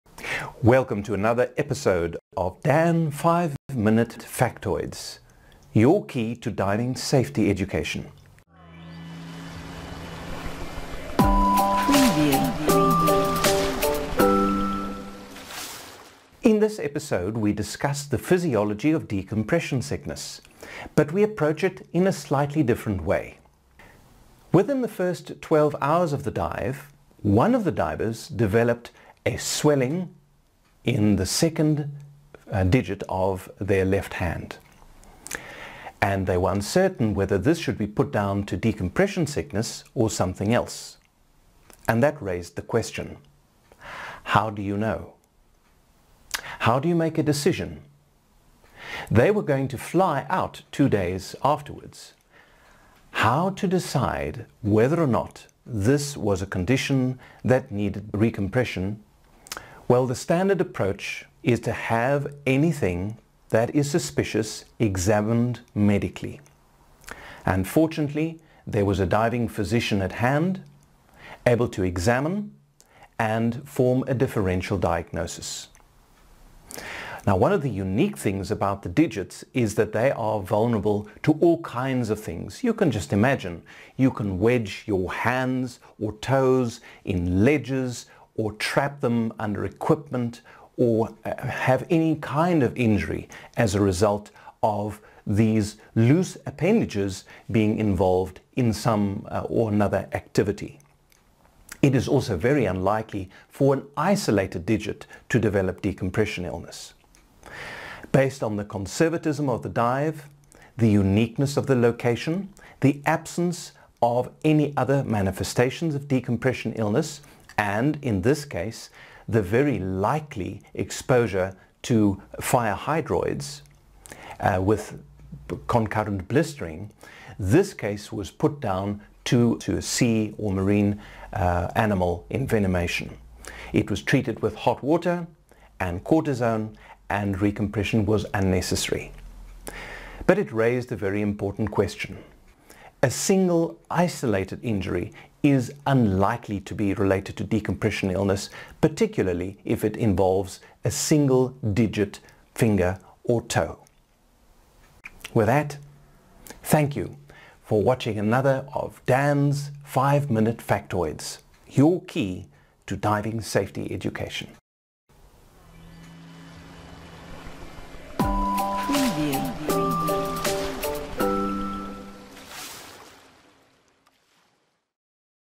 A new take on decompression sickness
Dr Frans Cronje talks about the physiology of decompression sickness.

Article list